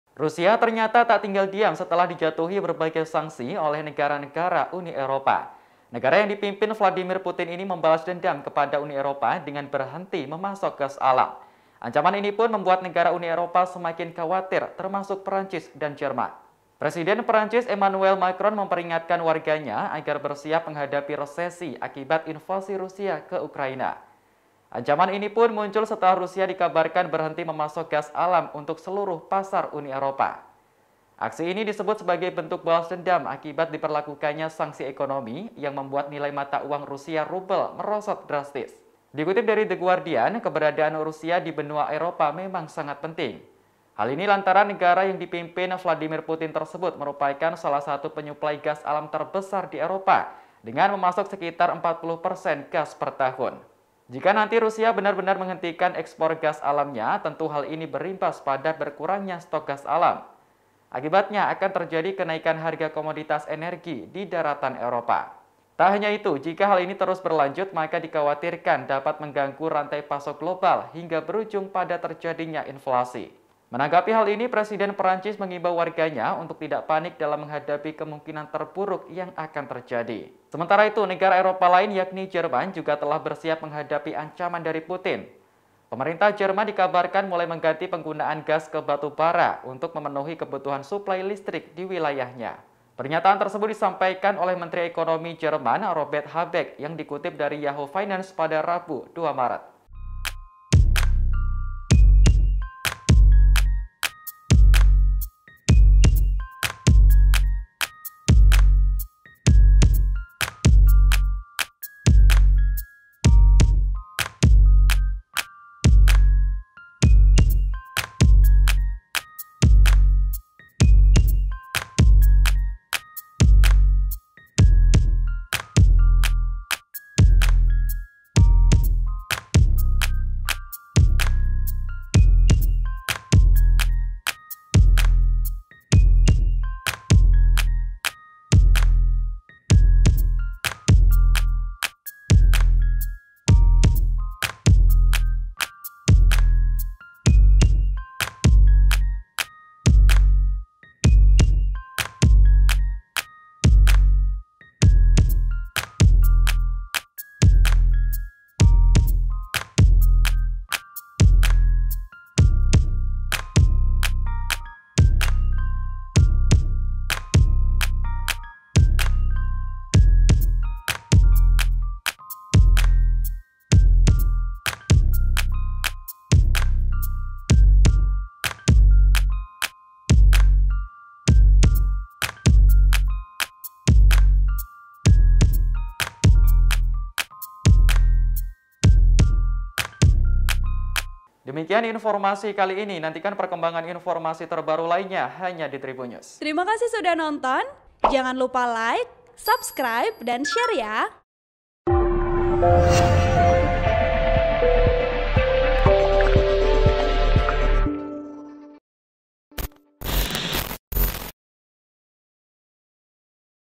Rusia Balas Dendam ke Uni Eropa, Ancam akan Stop Ekspor Gas Alam, Perancis dan Jerman Mulai Khawatir
TRIBUN-VIDEO.COM - Rusia ternyata tak tinggal diam setelah dijatuhi berbagai sanksi oleh negara-negara Uni Eropa.

Negara yang dipimpin Vladimir Putin ini membalas dendam kepada Uni Eropa dengan berhenti memasok gas alam.

Ancaman ini pun membuat negara Uni Eropa semakin khawatir, termasuk Prancis dan Jerman.